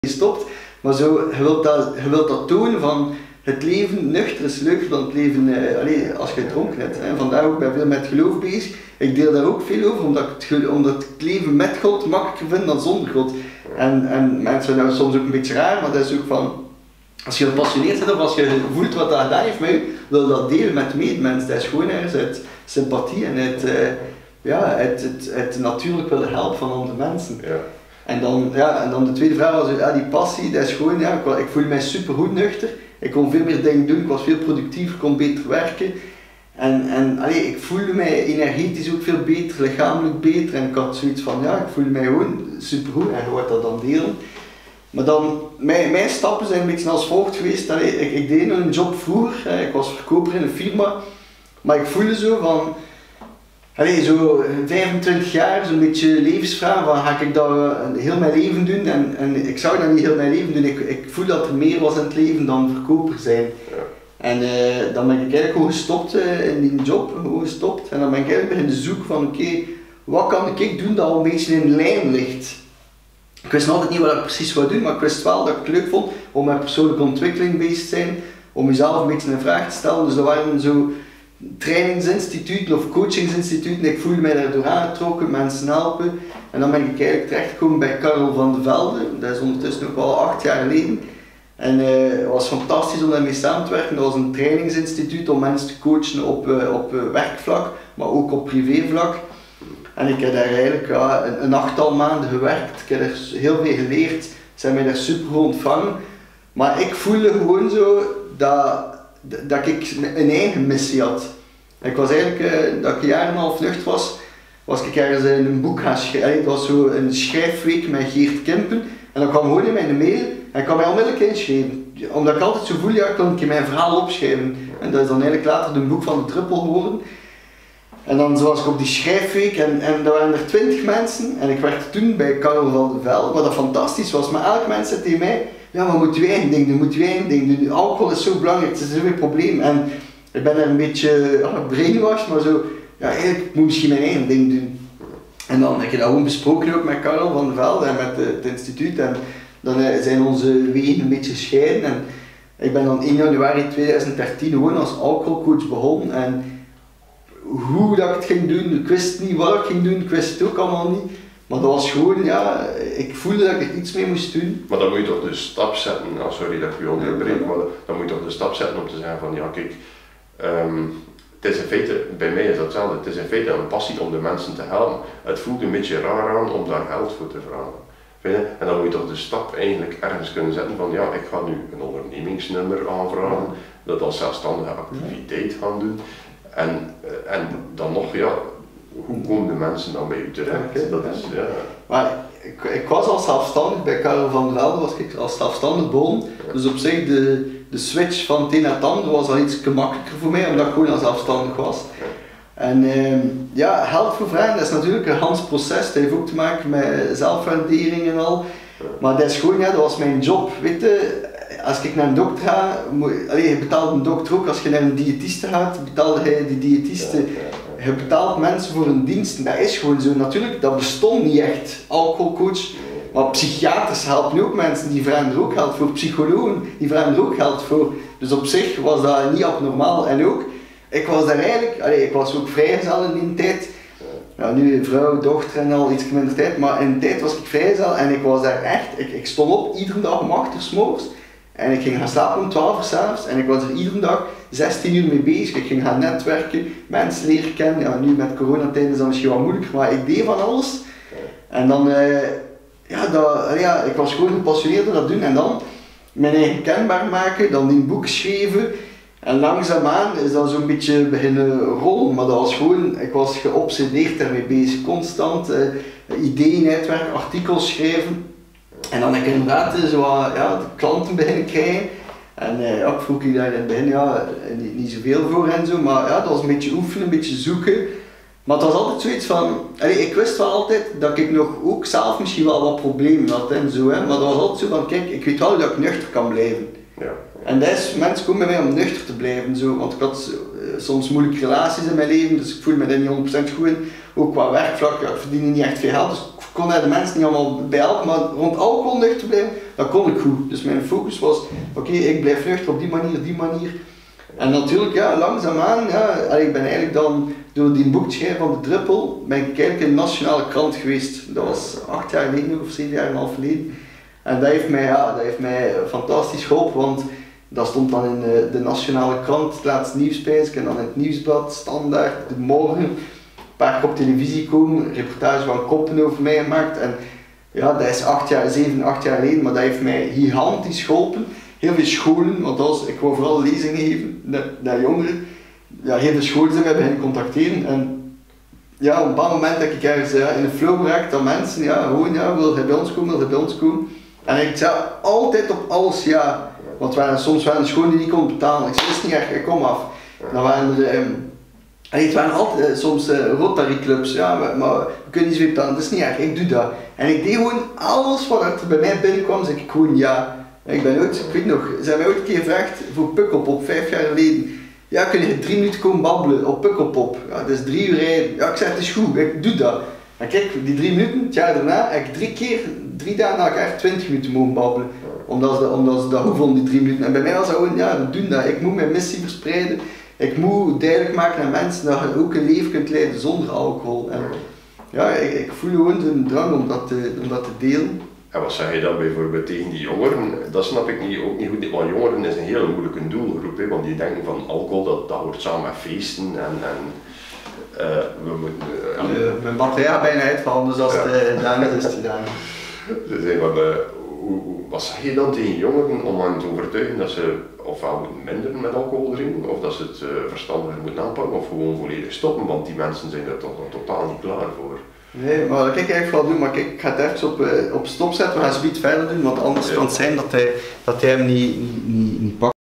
Deel 2 Interview met Mitch
Ik kreeg de vraag van volgende persoon: Op aanraden van mijn jobcoach die mij werd aangereikt  zou ik U willen vragen of U eventueel even tijd zou willen vrijmaken voor een verkennend gesprek in het kader van uw beroepsactiviteiten, om wat meer voeling te krijgen met Uw job en Uw passie, en om samen even te kunnen bespreken wat de hoofdzaken zijn die kenmerkend zijn voor een goede verslavingscoach, hoe U aan dit alles begonnen bent, en hoe U dit allemaal opgebouwd hebt.